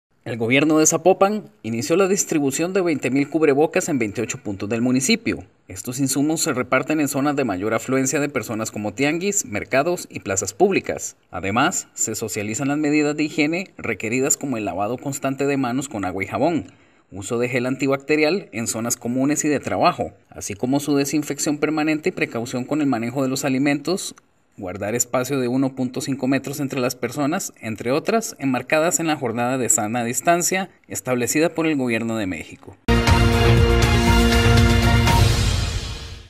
Refuerzan acciones preventivas
El Gobierno de Zapopan inició la distribución de 20 mil cubrebocas en 28 puntos del municipio. Estos insumos se reparten en zonas de mayor afluencia de personas como tianguis, mercados y plazas públicas.

Además, se socializan las medidas de higiene requeridas como el lavado constante de manos con agua y jabón, uso de gel antibacterial en zonas comunes y de trabajo, así como su desinfección permanente y precaución con el manejo de los alimentos, guardar espacio de 1.5 metros entre las personas, entre otras enmarcadas en la Jornada de Sana Distancia establecida por el Gobierno de México.

“Si el tema de la escasez no les permite adquirir los cubrebocas, el Ayuntamiento pueda proporcionarles esta herramienta para generarles condiciones de sanidad (…) (Queremos) invitar a la ciudadanía a que sea responsable, que genere condiciones de salubridad para su bienestar, para el bienestar de su familia y que juntos podamos construir comunidad y ser responsables con los demás”, explicó Sandra Vizcaíno Meza, Coordinadora General de Construcción de Comunidad.